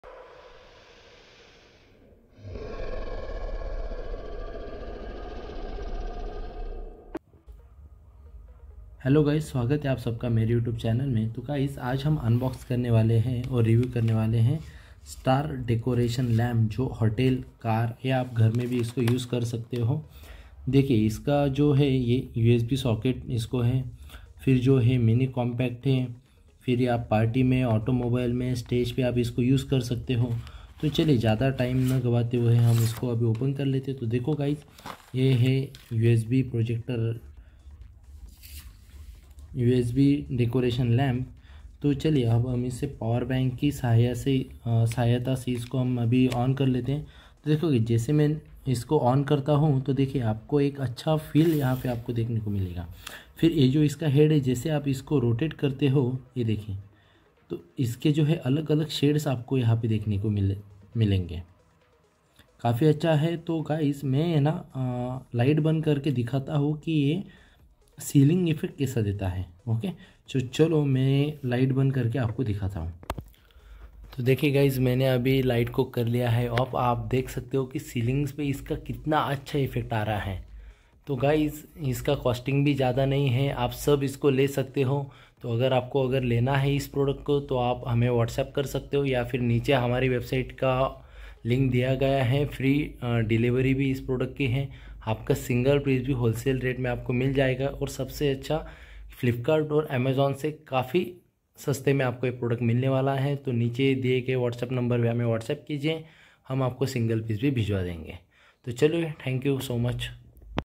USB LED LIGHT STAR PROJECTOR UNBOXING
Transform your spaces into a celestial wonder with our USB Star Projector—an enchanting device designed to bring the magic of the night sky to your home, car, or hotel room. Immerse yourself in a celestial experience with this compact and versatile projector.